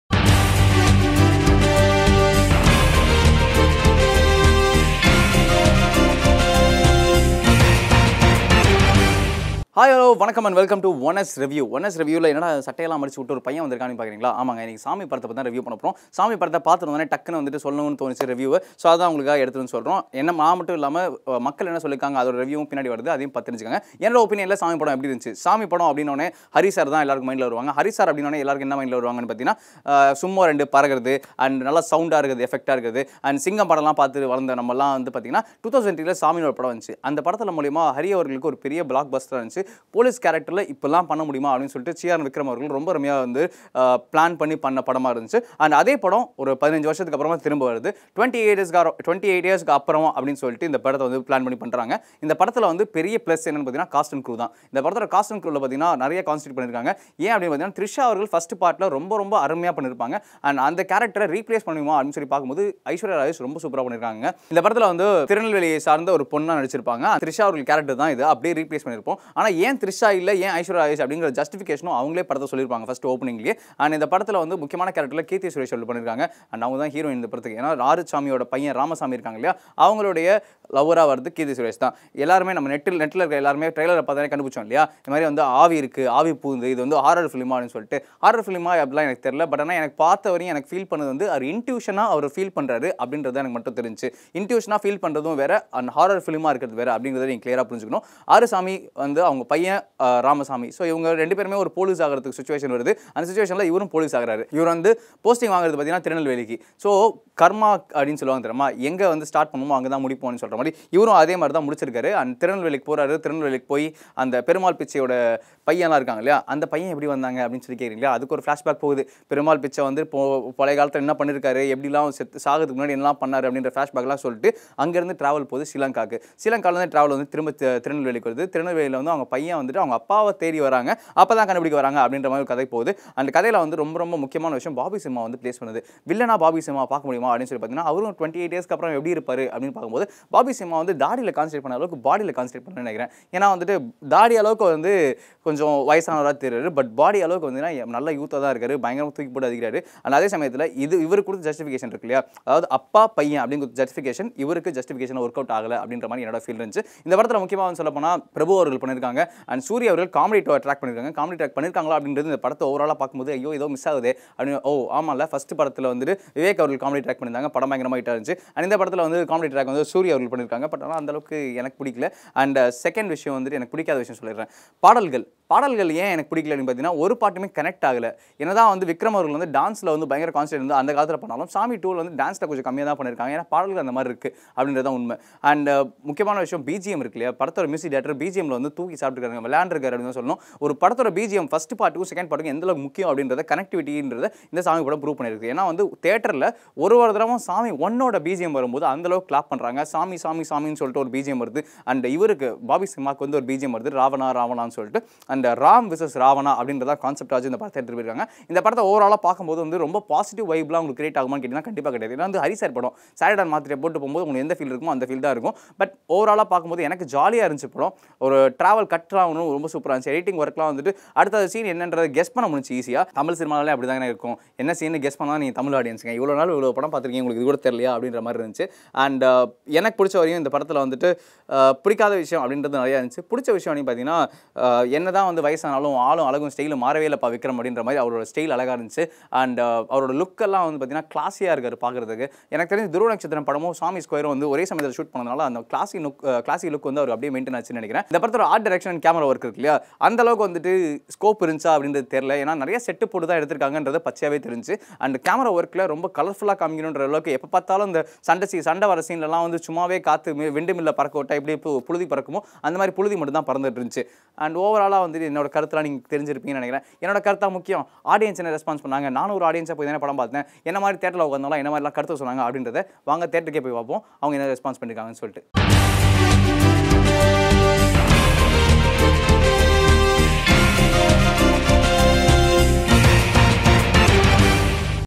Saamy 2 Movie Review | Chiyaan Vikram, Keerthy Suresh | Hari | Devi Sri Prasad | Yes Media
SaamySquare , the much –awaited sequel to the 2003 blockbuster Saamy directed by Hari. Starring ChiyaanVikram , produced by Shibu Thameens, Saamy Square has music by rock star DSP!